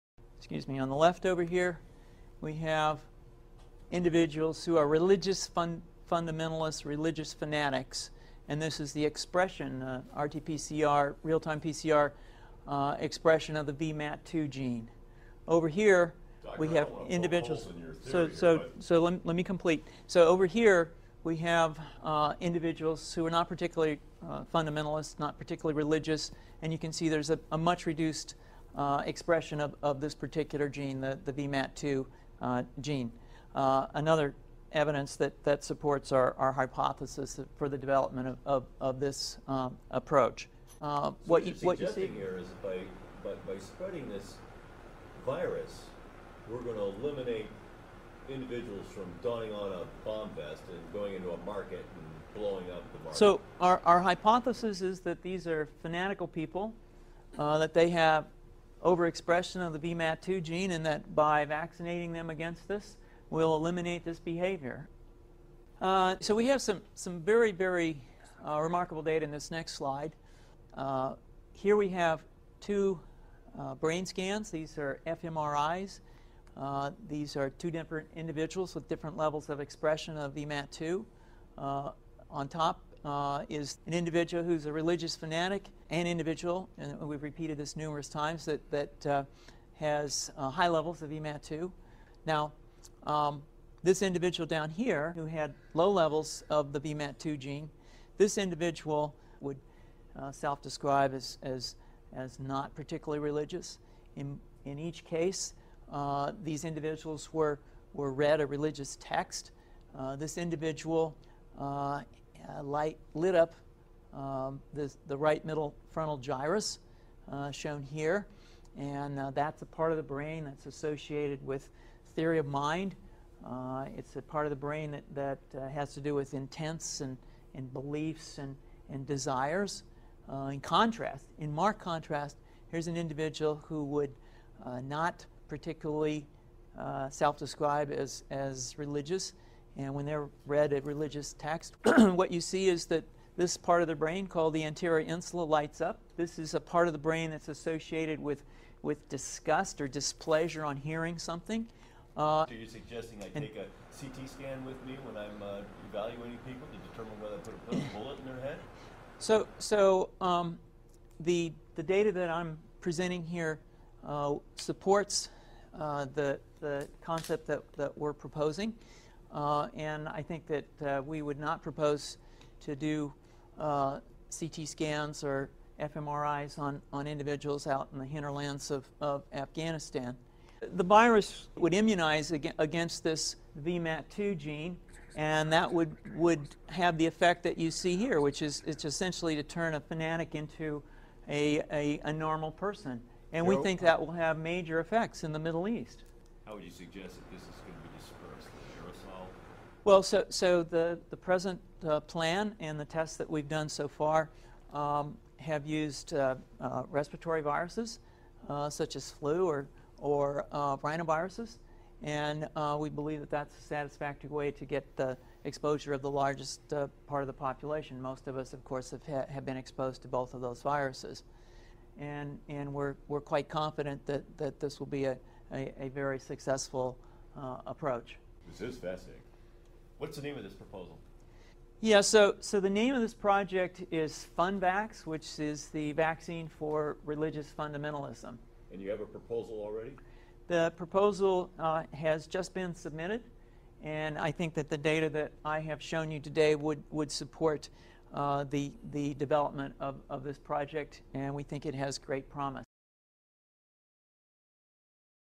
Pentagon Video Leaked Briefing on FUN-VAC ! 2005
First thing you need to do is to watch in 720pHD and with full scree so you can watch the details!

Some parts of the video the voice is ok, and then in other parts the voice is clearly over what the guy is saying, and the body movement is completely wrong. More, The video was edited! You can notice in some parts pixels hiding or trying to morph the mouth of the guy who is giving the lecture and the guy from the back row on the right. Some times the pixels hide the mouth movement to fake the speech in the recording and another time (the guy in the back row) the pixels are completely in a "bring to front" layer, hiding the mouth and jaw zone from moving.

At 2:21 when the supposed scientist coughs his body does not do the normal coughing movement, and from then on it really shows the displacement of the voice that is recorded on top of the film.

At 2:33 the same guy that asks those questions talks again, yet it does not look like anyone is running their mouth, and that voice came initially from the guy in the last row. From then on, the guy who is giving the lecture shuts up and then at 2:39 he says: "huhm...So.." and from this point on you can clearly see a voice over, even better was a couple of pixels were altered on the suppose lecturer mouth, watch it closely! Notice how the pixels in his mouth change slightly! That means the video was edited! Same thing seams to happen at 3:24 and beyond, this has not only been altered regarding the sound but the image itself. At 4:10 the guy who is in the last row near the camera on the right side, speaks and then shut up, but then wile the supposed scientist is talking, there seams to be a layered pixel in his jaw, pay close attention to his mouth and his jaw it looks like it was edited with layers to look like he is not talking and he makes a slight move with his body indicating he is talking., notice the same pixeling layers in the mouth of the suppose scientist! This is clearly a voice over and video editing scheme here.